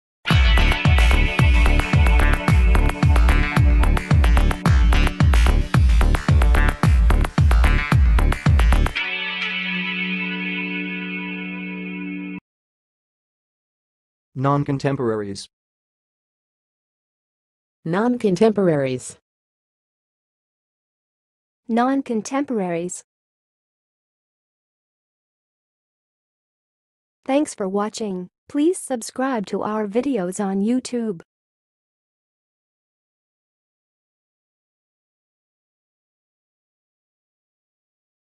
How to say "noncontemporaries"! (High Quality Voices)
Watch in this video how to say and pronounce "noncontemporaries"!